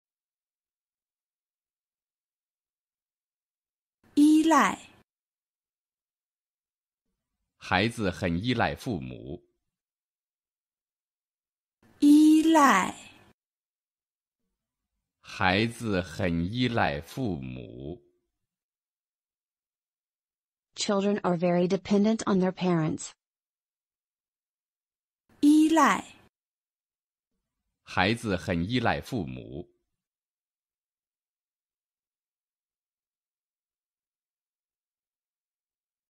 Chinese HSK 6 vocabulary 依赖 (yīlài), ex.1
In this video the chinese character is from HSK level 6. And it is "依赖", pinyin: "yīlài", which means "depend on; be dependent on".
Listen to native mandarin speakers saying "依赖" and the example: "孩子很依赖父母。", it means "Children are very dependent on their parents."